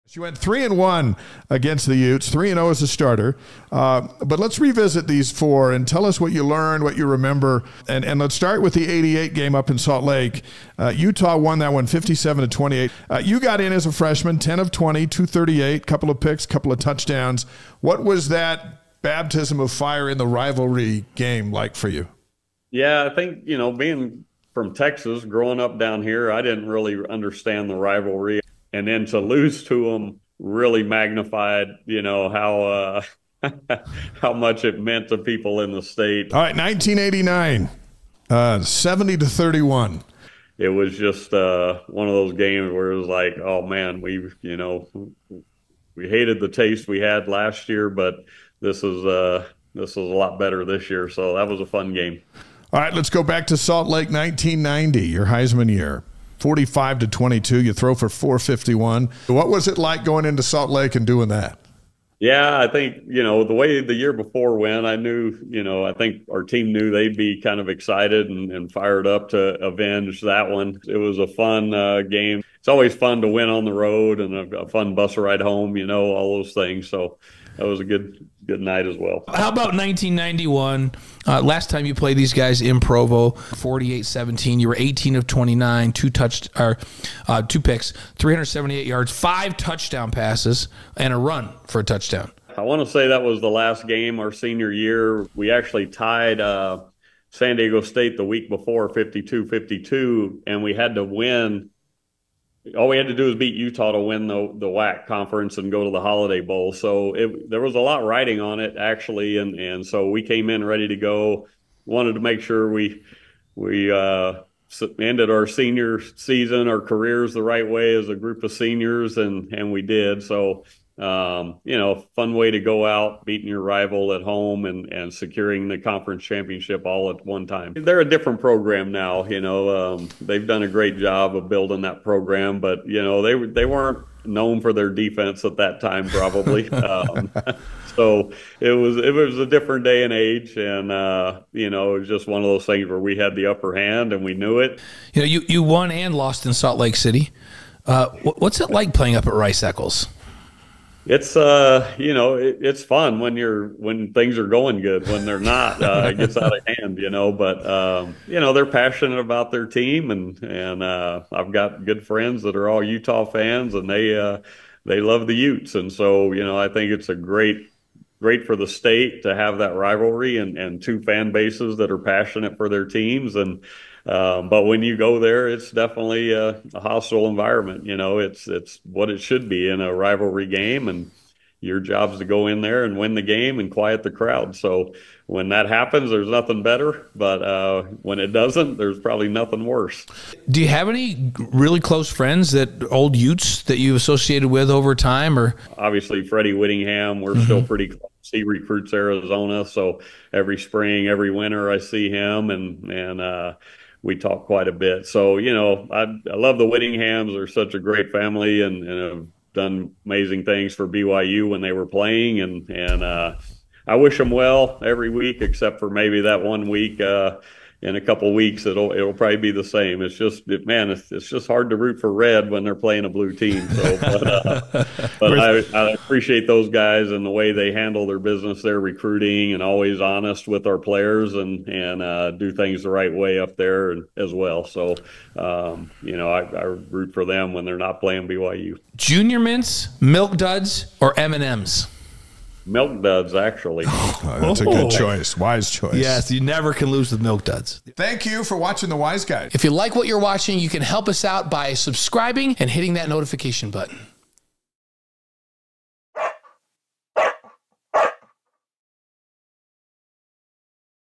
Ty Detmer Recalls His Four Rivalry Games Against Utah
During this short, 4-minute clip from this week’s livestream, Ty Detmer reflects on his four rivalry games against the University of Utah during his time at BYU, where he went 3-1.

Here’s a recap of each memorable matchup:

1988 (Salt Lake City) - Utah defeated BYU 57-28. As a freshman, Detmer got his first taste of the rivalry, going 10 of 22 for 138 yards, with 2 touchdowns and 2 interceptions. Despite the loss, he quickly learned how much this rivalry meant to fans in Utah.

1989 (Provo) - BYU crushed Utah 70-31. Detmer recalls the satisfaction of redeeming the previous year’s loss with a dominant performance at home. The game marked a turning point, solidifying his competitive spirit in this intense rivalry.

1990 (Salt Lake City) - BYU defeated Utah 45-22. In his Heisman Trophy-winning year, Detmer threw for 451 yards, leading BYU to a decisive victory on the road. He recalls the satisfaction of the bus ride back to Provo, celebrating a strong team win in a hostile environment.

1991 (Provo) - In his final game against Utah, BYU won 48-17, with Detmer passing for 378 yards, 5 touchdowns, and rushing for another. This win secured the WAC championship and a spot in the Holiday Bowl, making it a fitting finale to his rivalry career.

Ty reflects on the challenges of playing in Salt Lake City, where the rivalry atmosphere is intense and fans are fiercely loyal to the Utes. He shares his respect for friends on the Utah side, including the Whittingham family, while acknowledging that it’s always tough to root for Utah when they’re up against BYU.